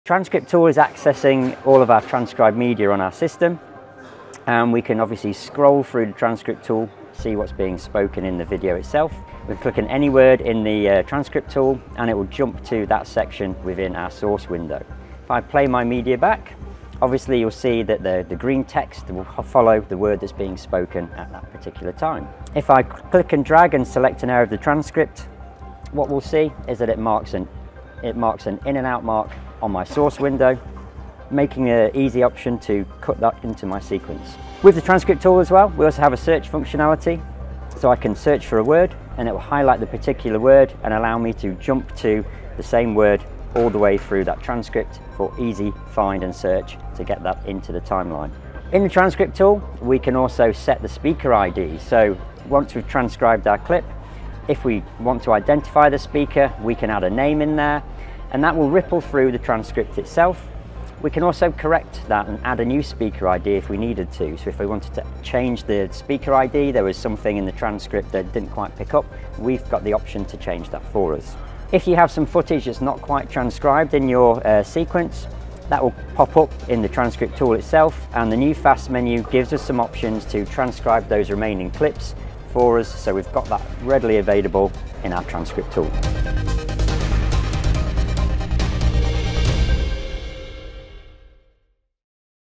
Avid Ada in Media Composer
Avid Solutions Specialist Alex Harbord highlights the key capabilities of the recently introduced Transcript window, powered by Avid Ada Phrasefind in Media Composer to enhance editorial workflows